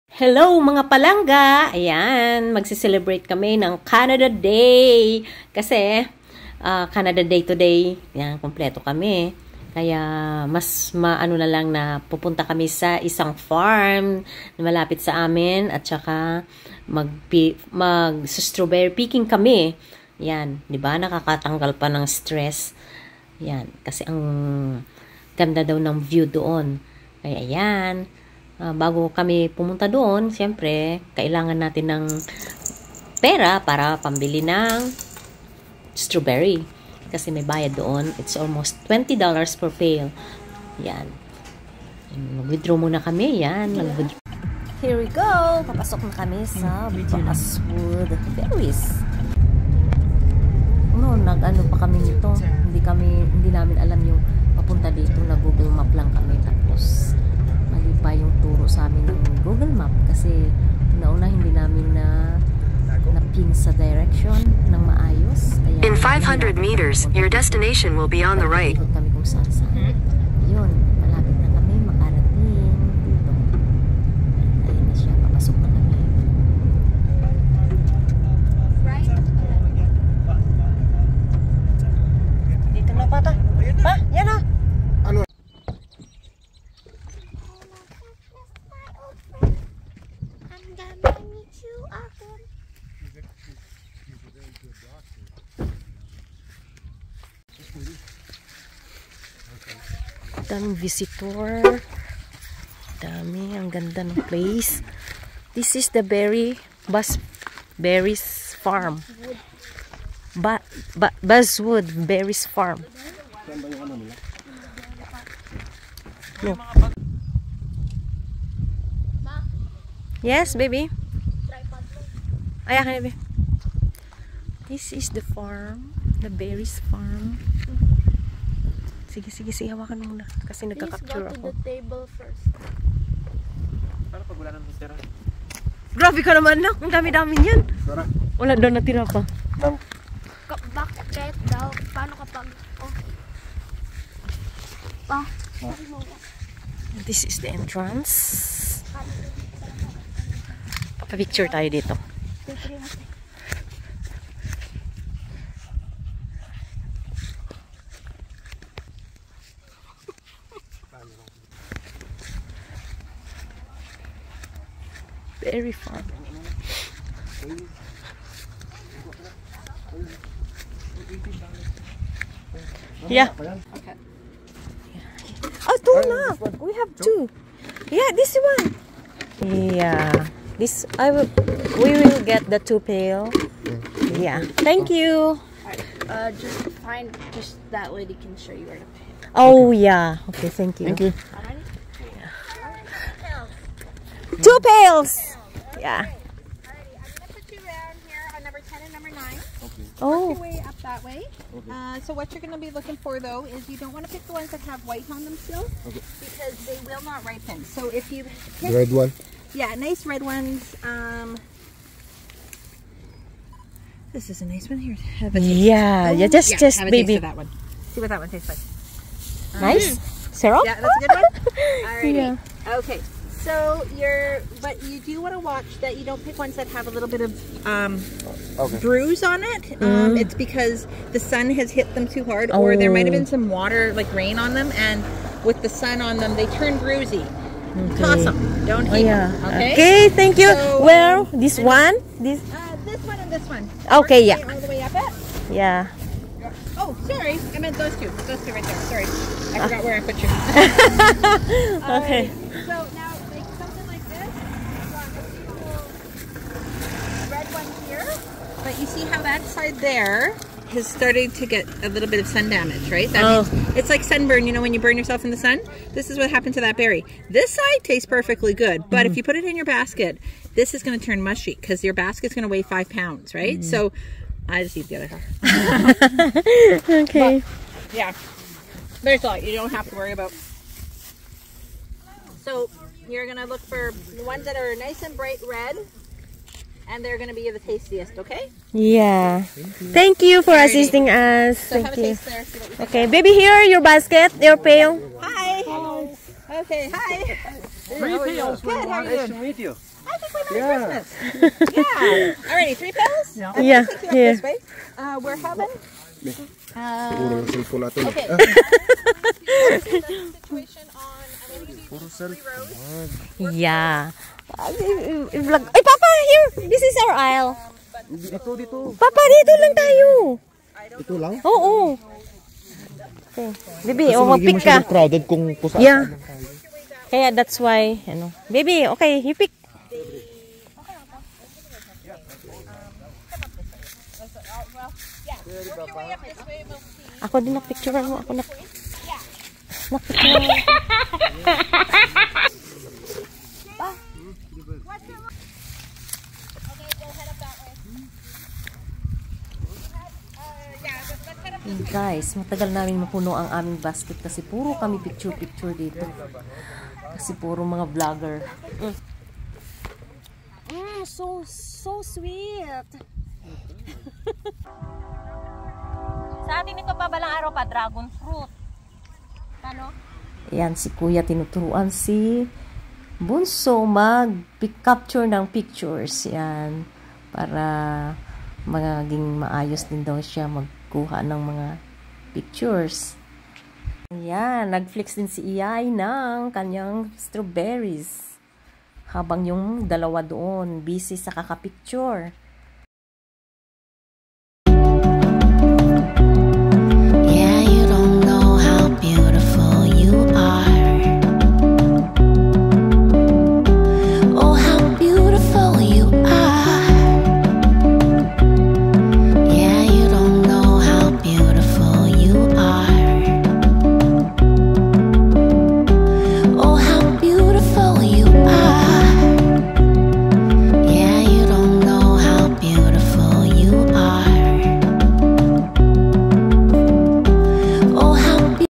STRAWBERRY PICKING IN CANADA| Part 1
It summer now in Manitoba Canada mag Strawberry picking kami dito sa Minnedosa at Basswood Berries Farm.